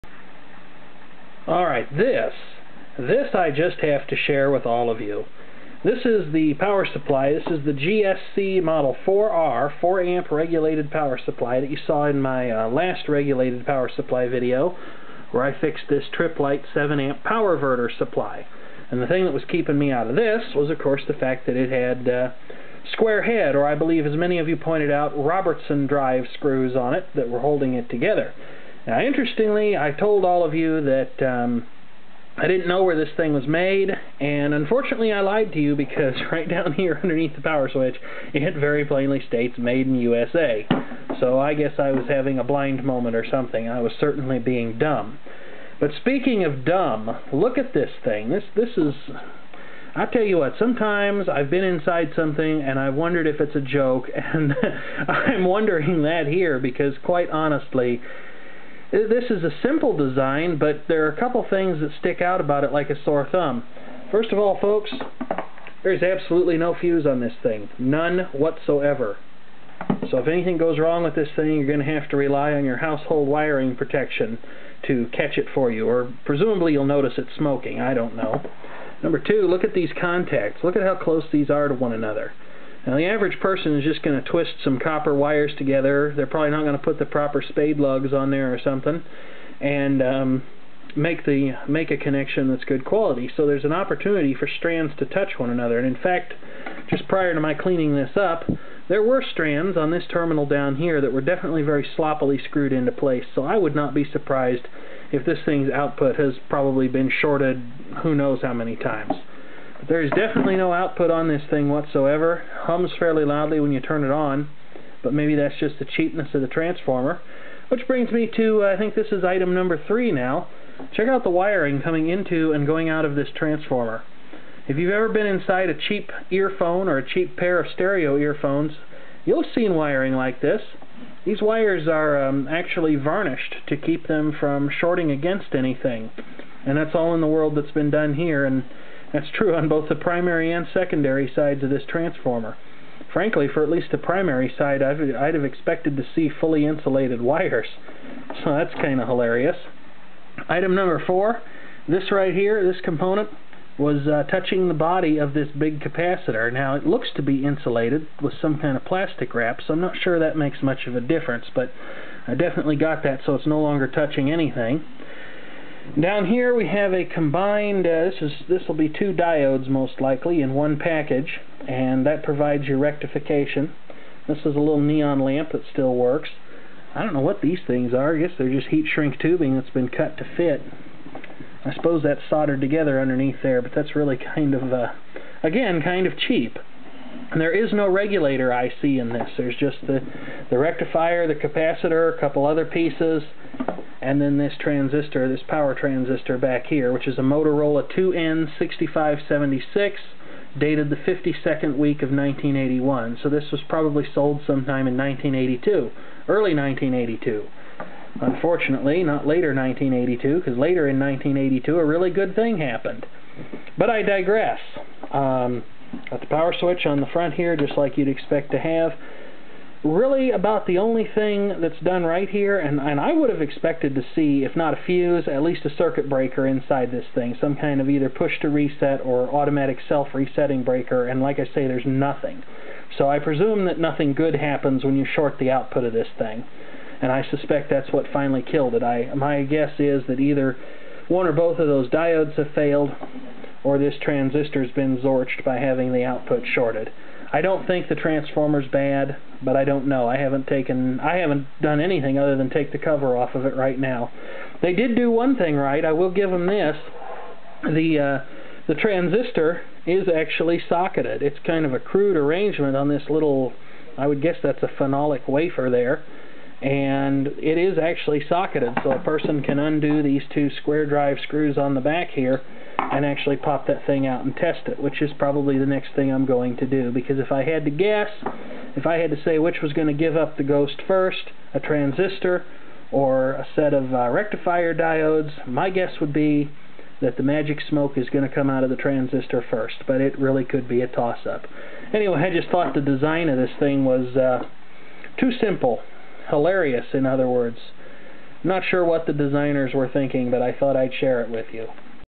Is This a Power Supply or a Joke?
We're done with the comments on this one. I lacked understanding of how the regulation worked. Thanks to those who were polite and professional in explaining how the regulation was meant to work. Those of you who felt that by being rude you'd somehow make your point indelible...well, you can go and learn some manners for next time. Or better yet, just go away.

The large Motorola transistor tested bad, was found to no longer be in production and this unit was ultimately parted out.